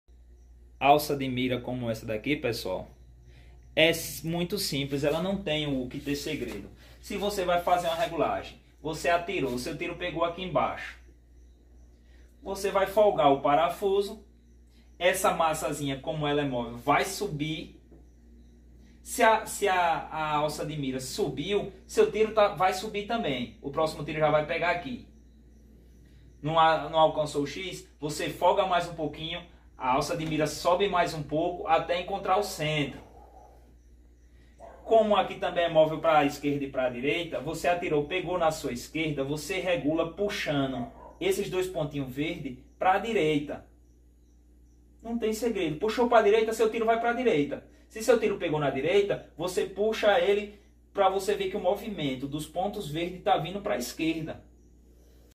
regular mira aberta facil
video completo de como regular facilmente uma carabina , icluindo apoio , distancias e mais